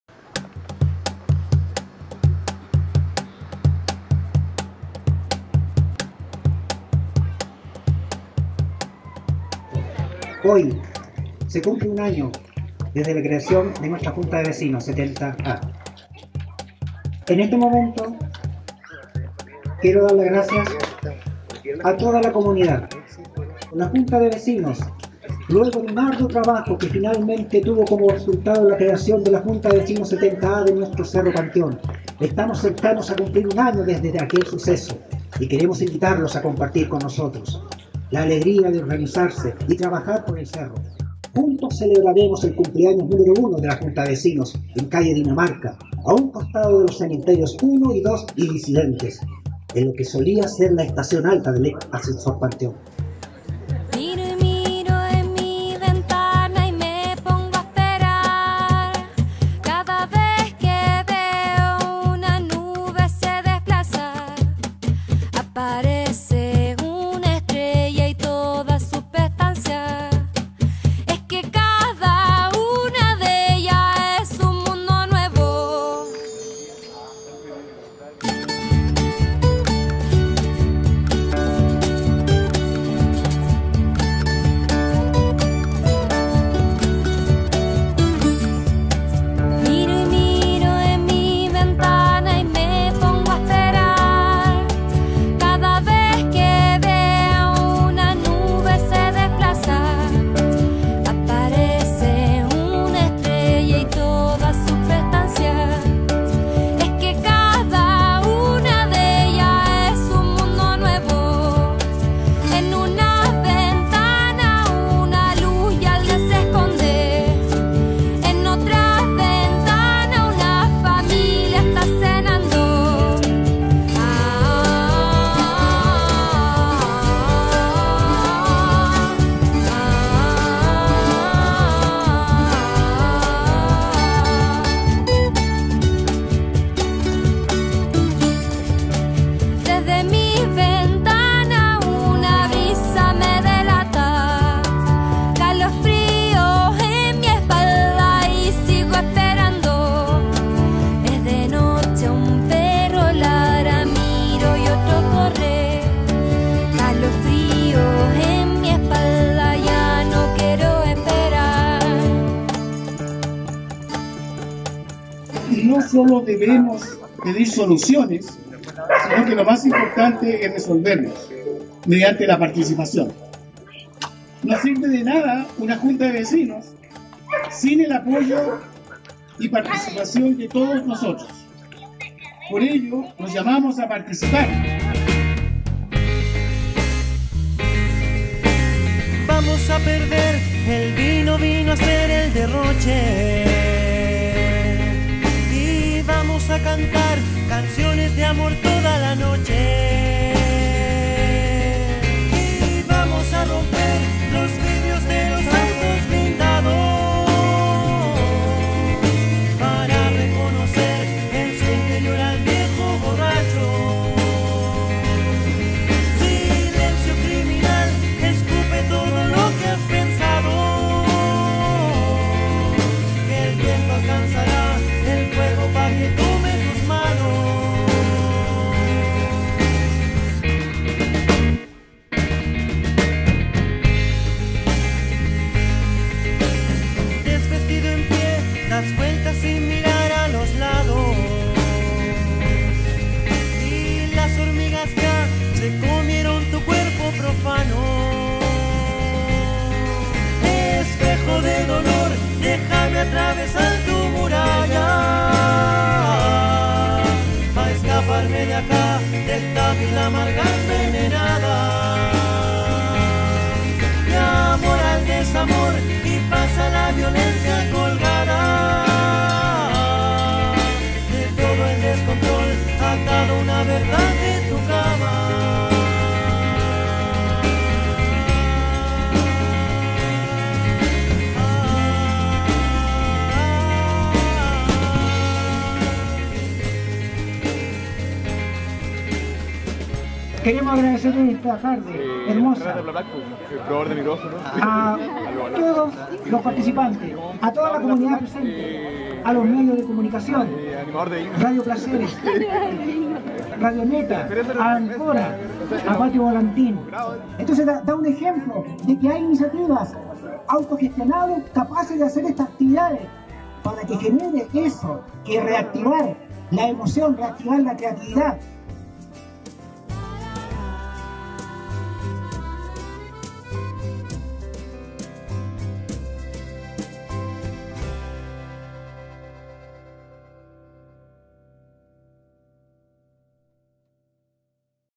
Primer aniversario JJVV Cº Panteón
Registro de actividades efectuadas el 27 de Julio en calle Dinamarca en el Cº Panteón, correspondientes al aniversario número uno de su junta vecinal.

Registro: Áncora, Post Producción: Alejandro Da Silva